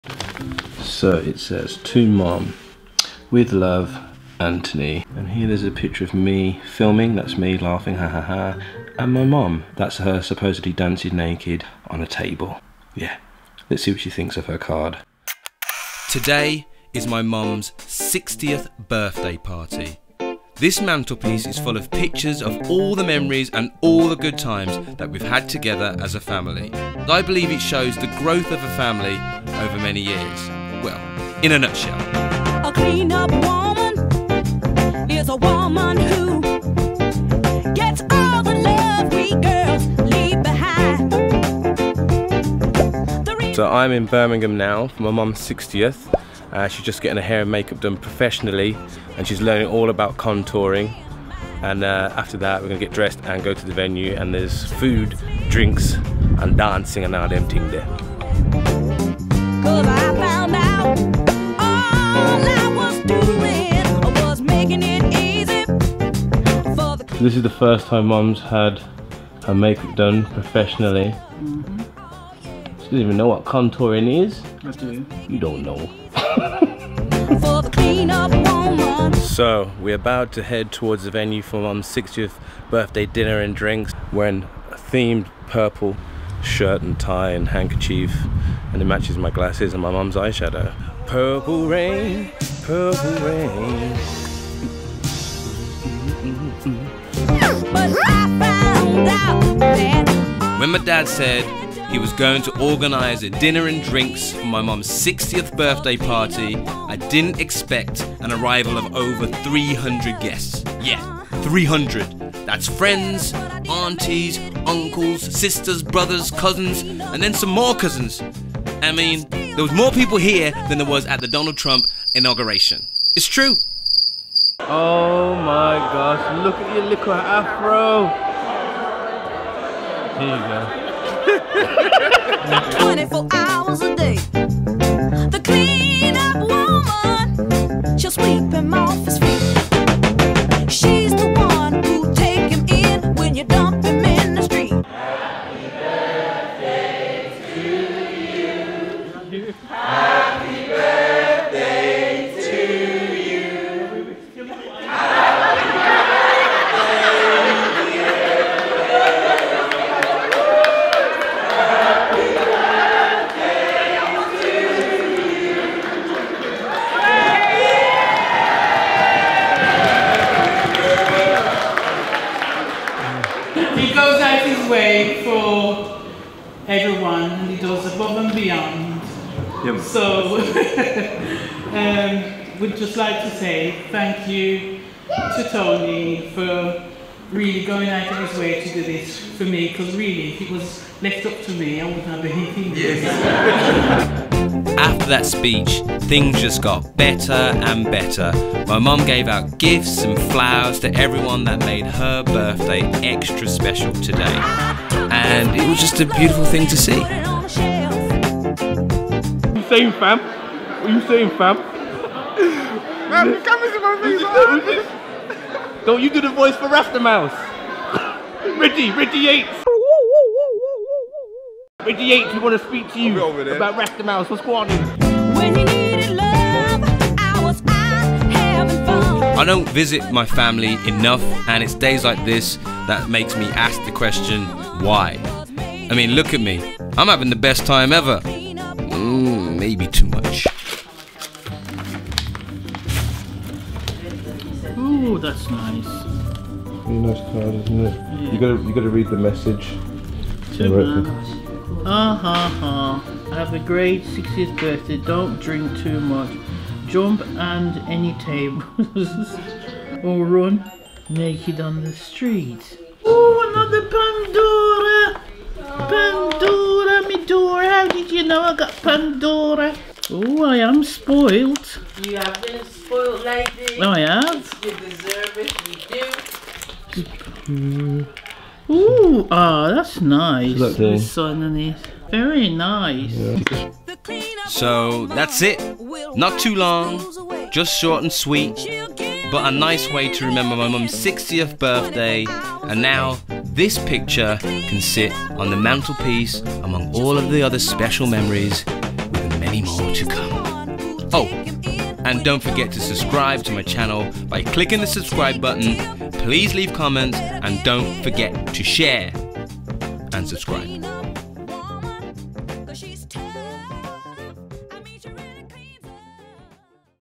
Mum's 60th Birthday Party
If you you have ever wondered how to celebrate your mum's 50th  60th or even 100th. Why not make a documentary of the day to remember it by. You can film it just the way I have here for my mum's 60th birthday. Enjoy!

Being Mr Brown